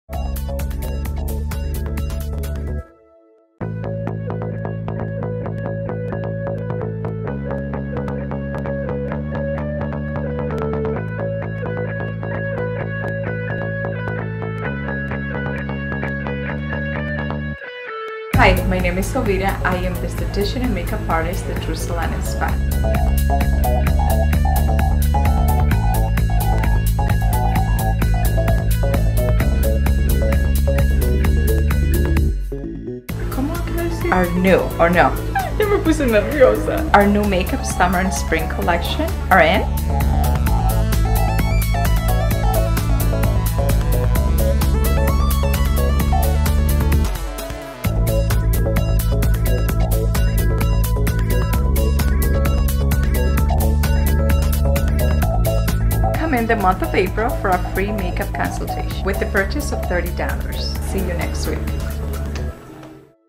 Girl Shaves Her Head For St. Baldrick's & Gets A Spring Makeover!
Corrina, a beautiful and brave girl shaves her head for St. Baldrick's, a charity that funds research for childhood cancer. Take a look at her new makeover as Jovita combines Corrina's darker style with a bright touch of spring.

Stop by Tru Salon & Spa this April for a free makeup application with your purchase of $30 in makeup.